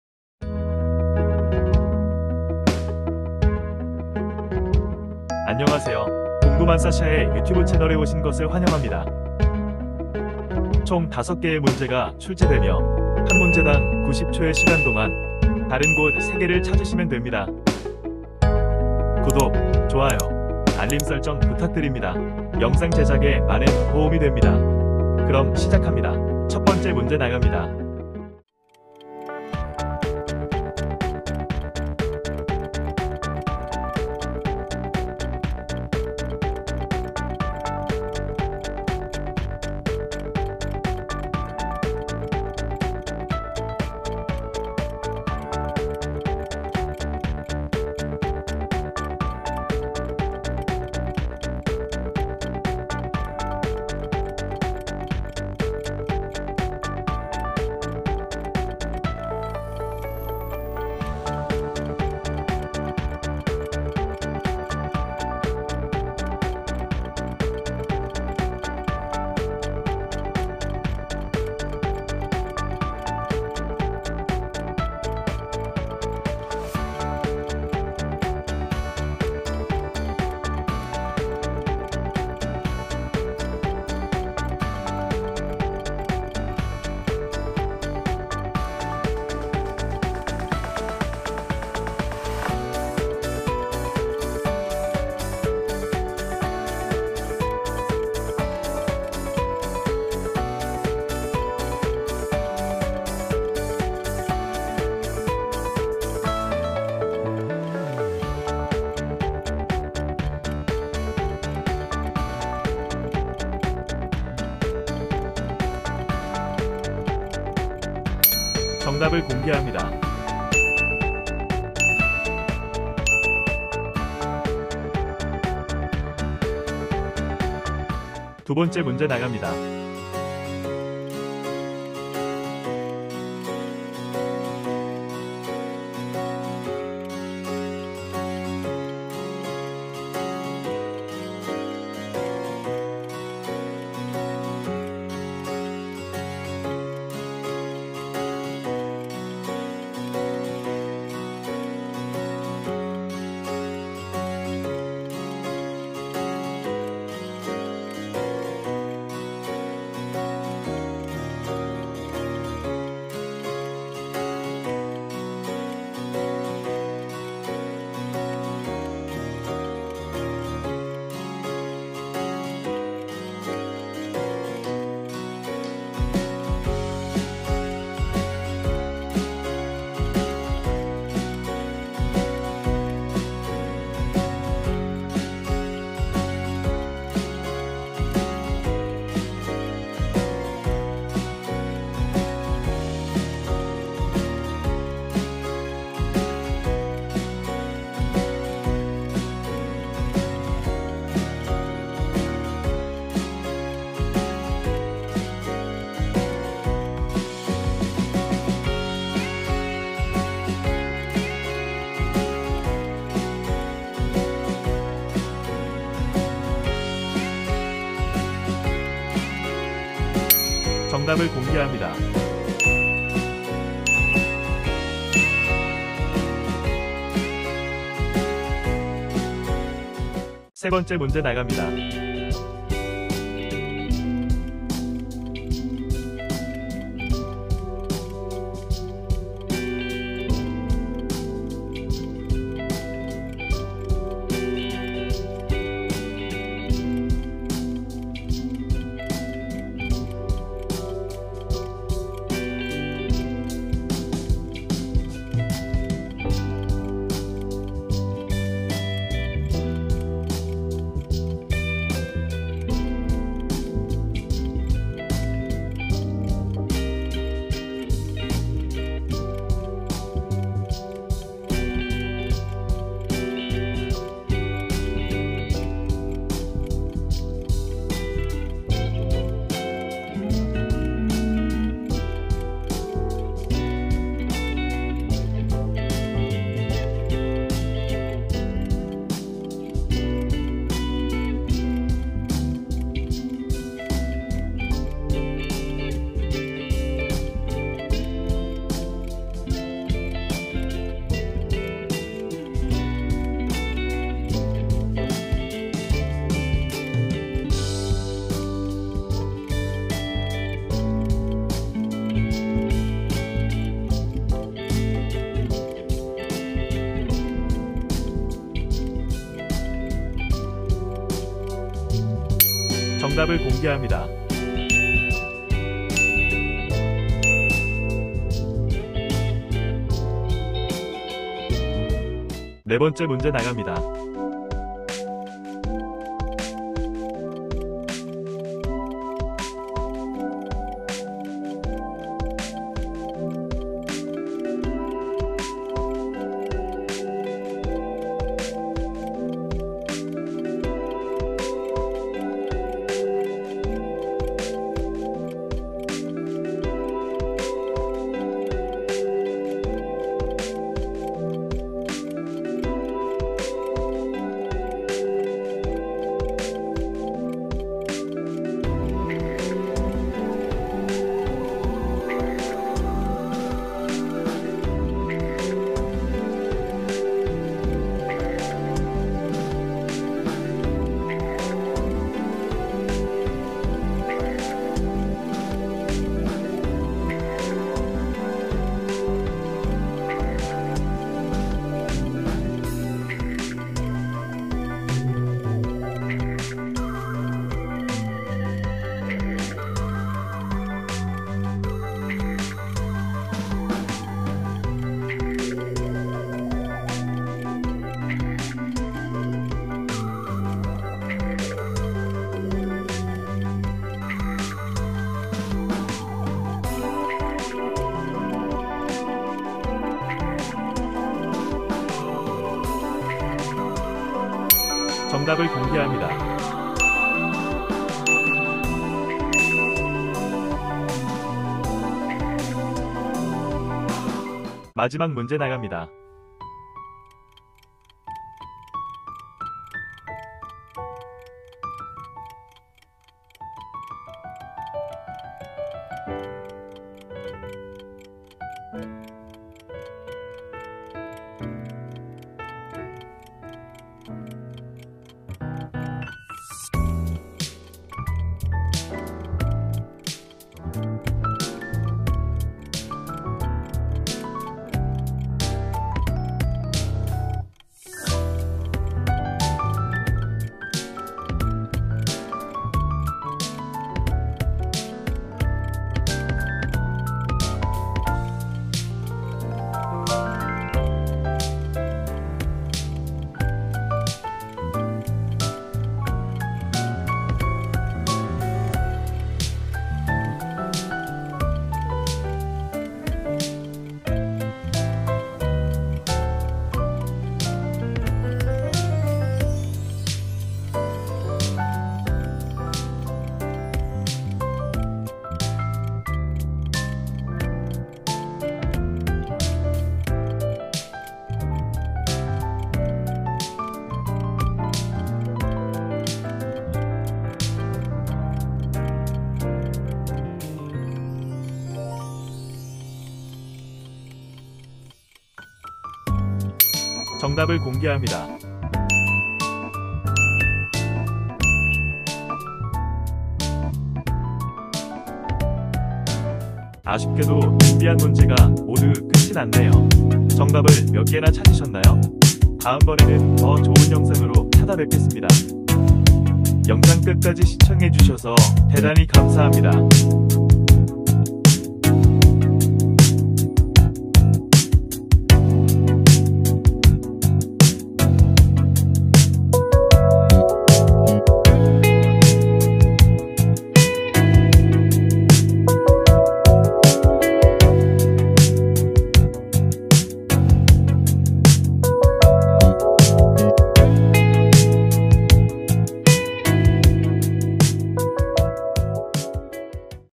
EP.024 스튜어디스 - 영상 [브레인연구소] [브레인 집중력 퀴즈 다른그림찾기 틀린그림찾기 치매예방 그림 사진]
90초안에 다른 곳 3개를 빠르게 찾아보세요.
총 다섯 단계의 문제가 준비되어 있습니다.
그럼 행운을 빌어요!
✅ 다른그림찾기(spot the difference)
두개의 그림을 비교하여 서로다른부분을 찾는 퍼즐게임으로
집중력, 기억력, 관찰력, 인지력 향상에 도움을 주는 게임입니다.
0:00 다른그림찾기 OP
00:26 1번 문제
01:58 1번 문제 [정답]
02:08 2번 문제
03:40 2번 문제 [정답]
03:50 3번 문제
05:22 3번 문제 [정답]
05:32 4번 문제
07:04 4번 문제 [정답]
07:14 5번 문제
08:46 5번 문제 [정답]
08:56 다른그림찾기 ED

ℹ️ Info.
⚙️ 편집 : CapCut, Canva, Photoshop, Dall-e3, ChatGPT
🎼 음악 : CapCut
🎤 음성 : CapCut